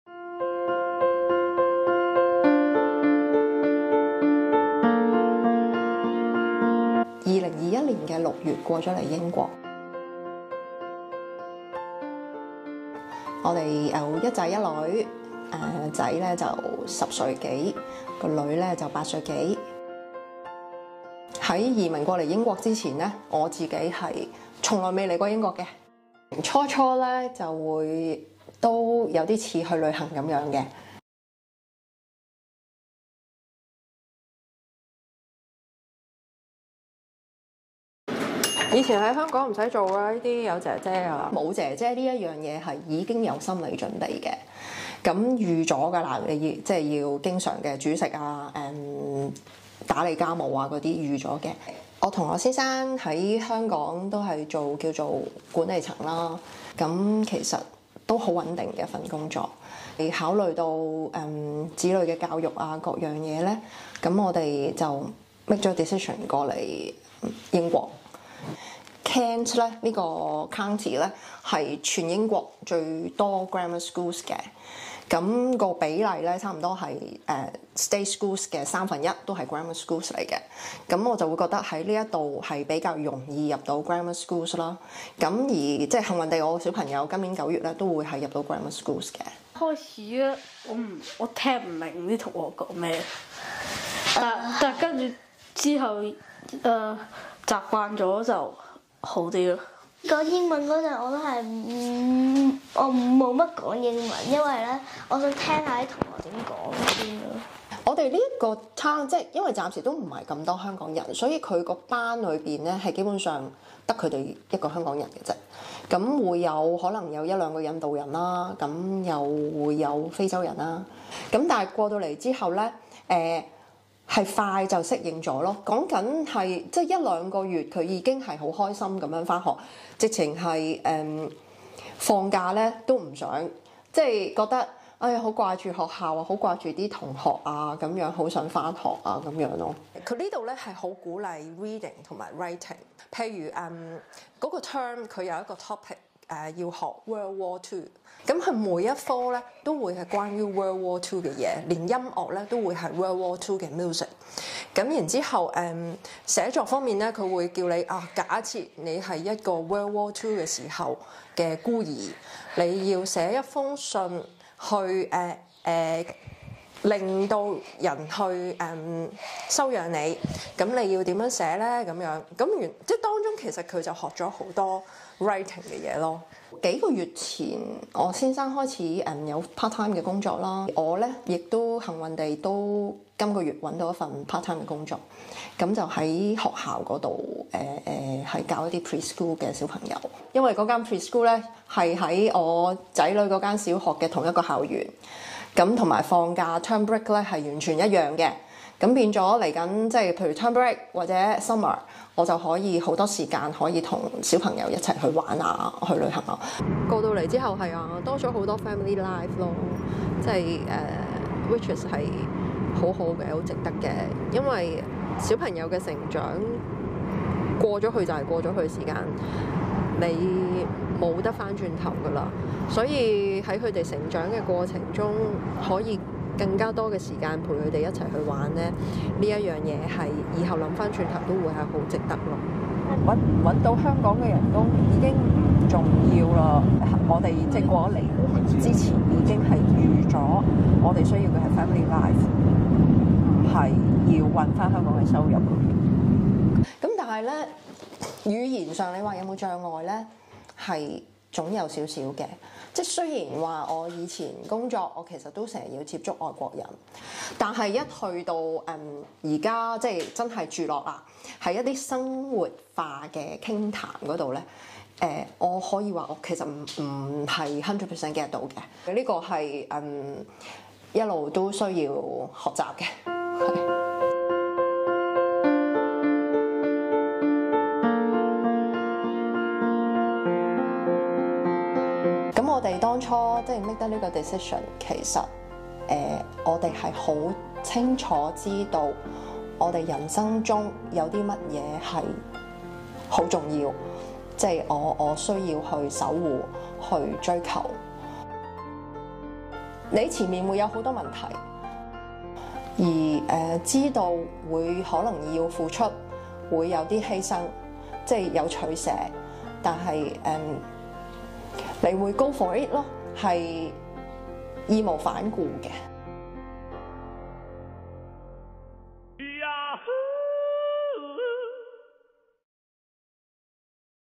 【香港人在英國】昔日公司管理層放下事業 追求家庭生活重新開始｜Yahoo Hong Kong
【Yahoo 新聞報道】Ally 一家四口抵英一年有多，現居英格蘭的肯特郡（Kent）。Ally 跟丈夫在香港本來都是公司管理層，工作穩定，但他們都寧願放下香港的事業，到英國重新開始；Ally 說得清楚，她們追求的是家庭生活，而且希望更專注於子女：「細路仔嘅成長過咗去就過咗去，無得返轉頭」。實際上，兩位小孩也很快適應當地生活，一兩個月已經融入校園，甚至不想放假。雖然移居他方有不少問題需要克服，但 Ally 都表示，只要認清自己的目標，「你就會大膽走出去，義無反顧。」